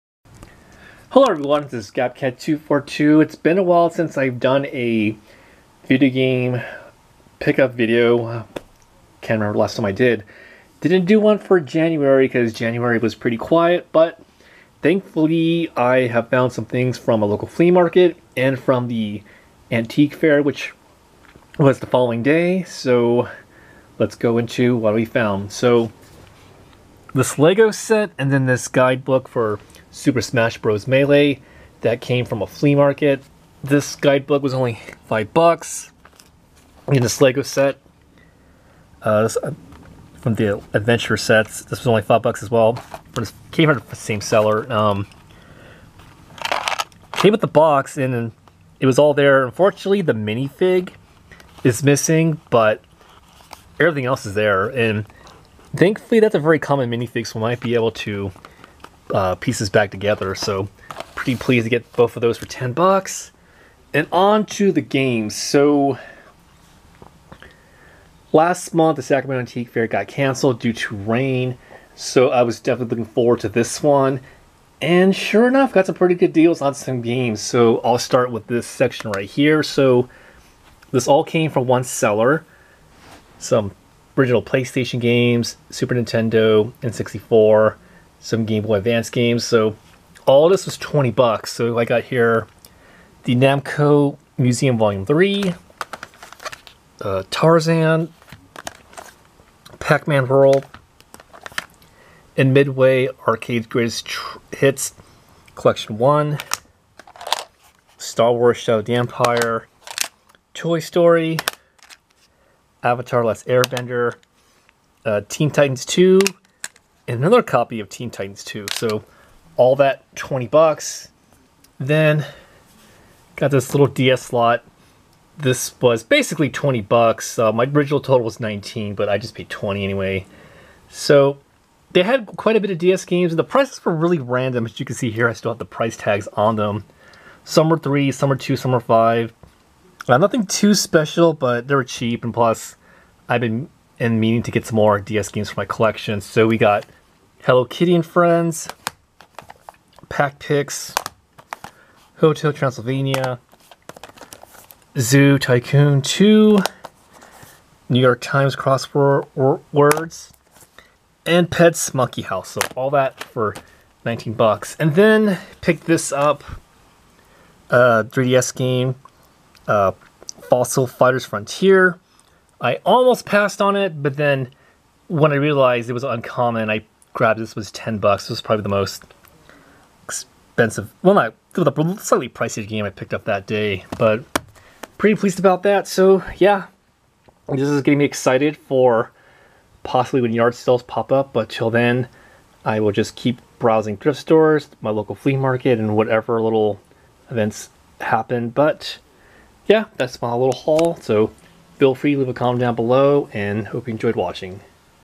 Video Game Haul February 2024
I finally found some stuff. I went to my local flea market and the sacramento antique faire last weekend.